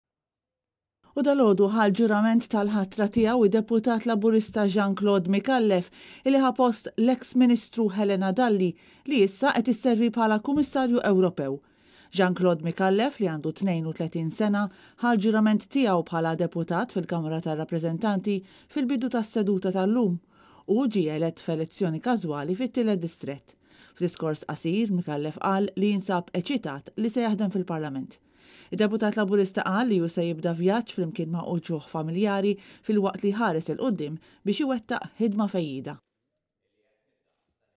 Jean Claude Micallef fil-Parlament
Dalgħodu ħa l-ġurament tal-ħatra tiegħu d-deputat Laburista Jean Claude Micallef, illi ħa post l-eks Ministru Helena Dalli – li issa qed isservi bħala Kummissarju Ewropew.